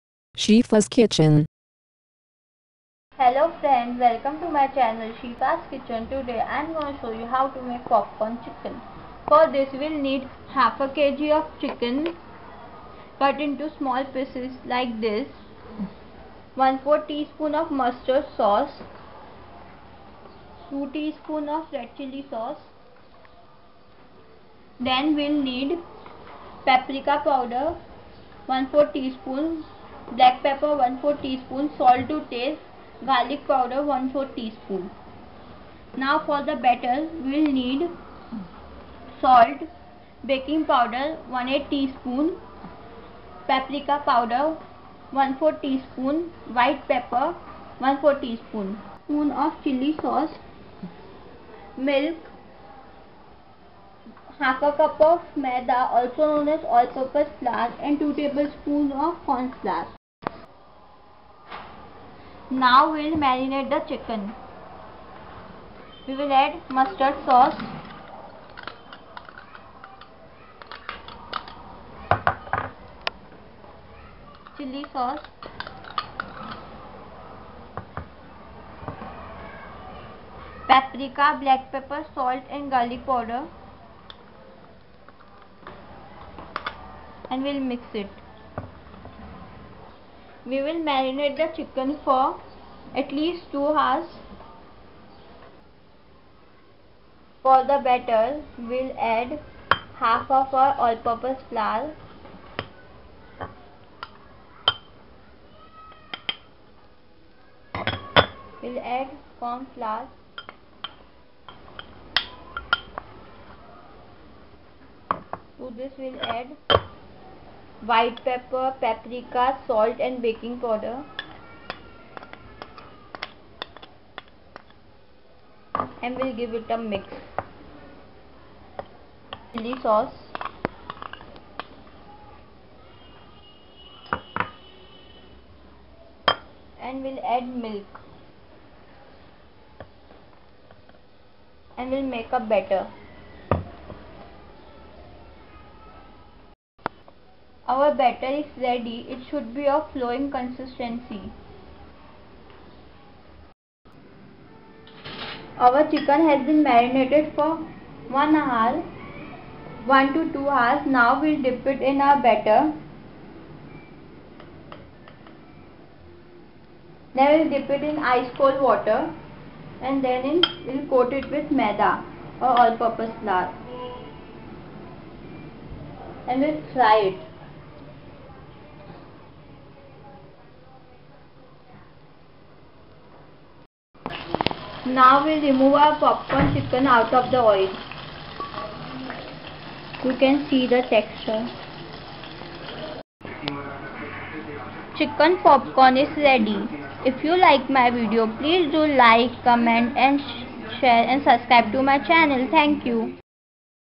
** KFC STYLE CHICKEN POPCORN
the secret is here kfc style popcorn chicken very tasty and yummy without any preservatives .

** VEG NOODLES **

**SPONGE CAKE**

 **RICE CHAPATI**

**BOHRI BIRYANI**

XOXO ,
SHIFA'S KITCHEN